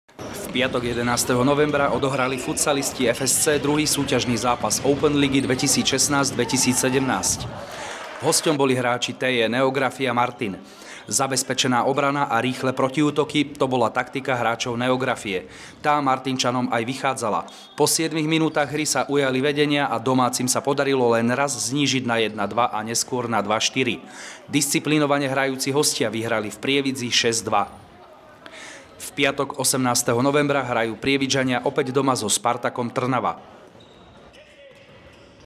Ani druhý pokus sa futsalistom nevydaril
V piatok 11.novembra odohrali futsalisti FSC druhý súťažný zápas Openligy 2016/1207. Hosťom boli hráči TJ Neografia Martin. Zabezpečená obrana a rýchle protiútoky, to bola taktika hráčov Neografie.. Tá  Martinčanom aj vychádzala. Po siedmich minútach hry sa ujali vedenia a domácim sa podarilo len raz znížiť na 1:2 a neskôr na 2:4. Disciplinovane hrajúci  hostia vyhrali v Prievidzi 6:2.
V piatok 18.novembra hrajú Prievidžania opäť doma so Spartakom Trnava.

FSC Prievidza - TJ Neografia Martin 2 : 6 (1:3)
počet divákov :100
rozh.: Behančín , Matula
Góly:  Madarasz 2 - Špirko 3, Kucej, Doboš, Struhár